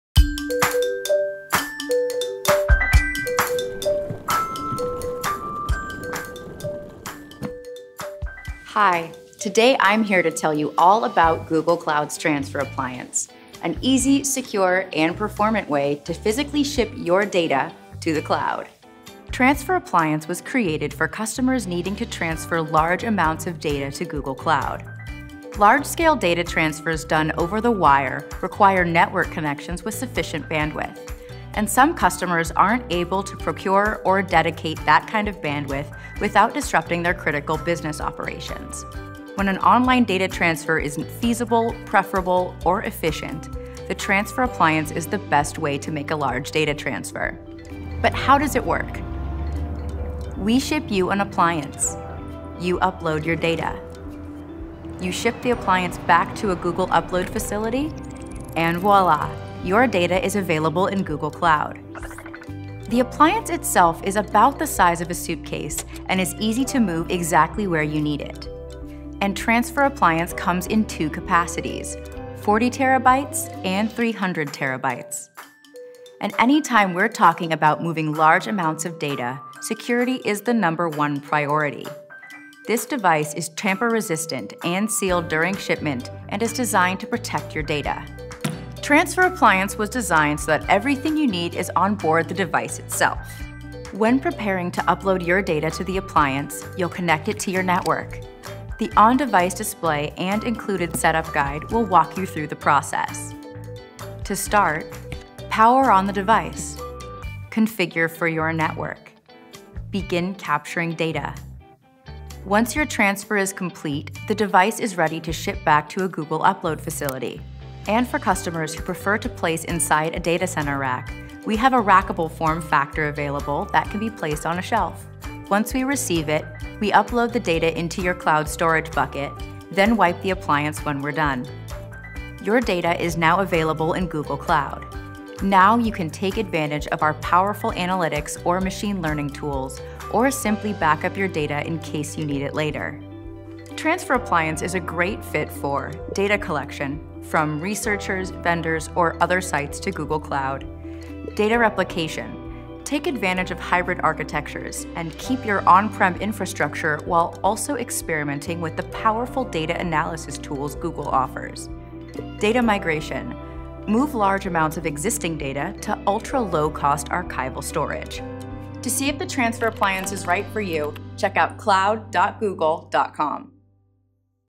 Introducing Google Cloud’s Transfer Appliance
Google Cloud’s Transfer Appliance offers an easy and secure way to physically ship large amounts of your data to the cloud. In this video, we show how Transfer Appliance is useful for data collection, replication, and migration. Watch to see if Transfer Appliance is right for you!

product: Cloud - Migration - Transfer Appliance; fullname: Jenny Brown;